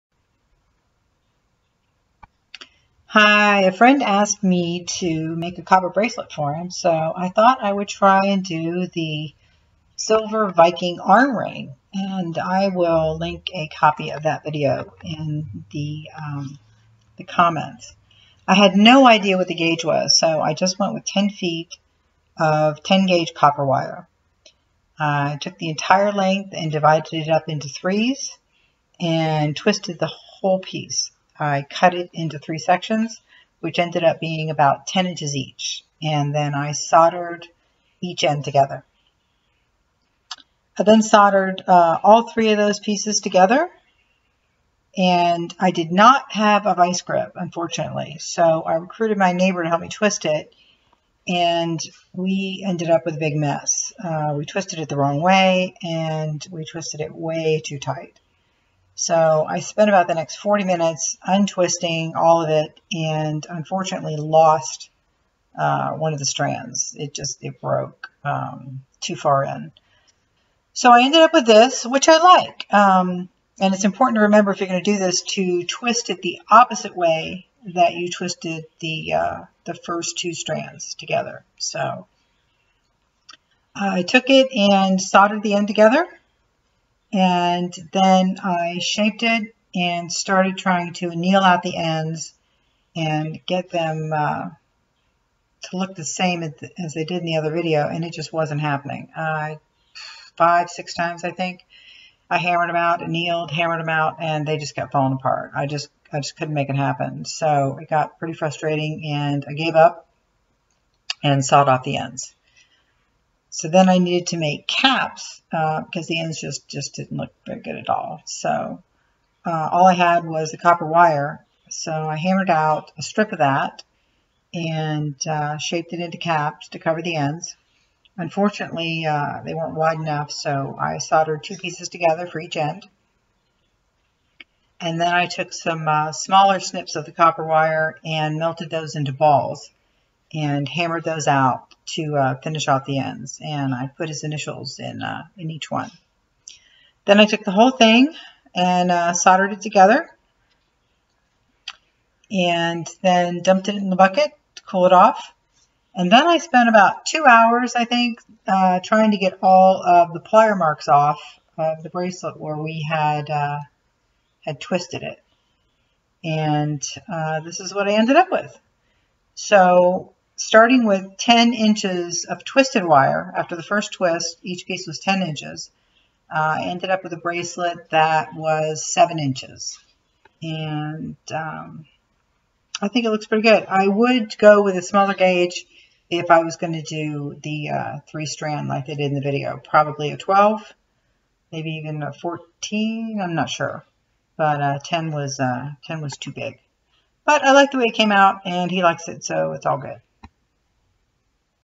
How to Make a Viking Bracelet - Modified in Copper
Viking Bracelet - Modified

If I were to do it again, and wanted 3 twisted strands,  I'd use a 12 gauge wire and I'd get a vice instead of using my neighbor. I still don't know how they got the long ends hammered out without it falling apart. At the end the bracelet was 7"